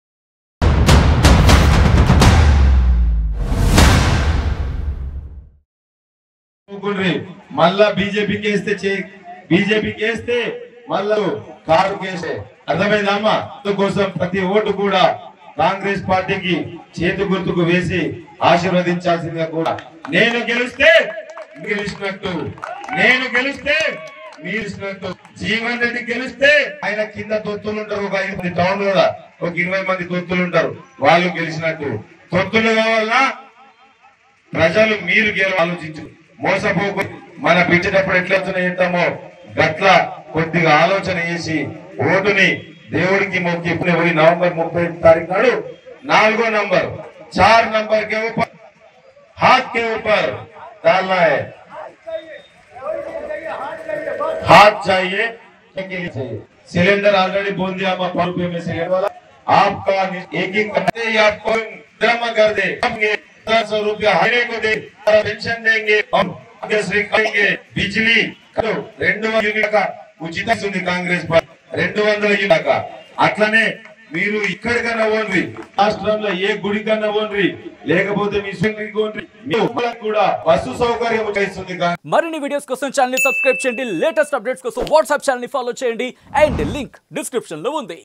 Congress Leader Vinay Kumar Reddy Sensational Comments | కేసీఆర్ చెప్పే మాయ మాటలకు మోసపోకండి..|| RTV
Congress Leader Vinay Kumar Reddy Sensational Comments | కేసీఆర్ చెప్పే మాయ మాటలకు మోసపోకండి..|| RTV Nizamabad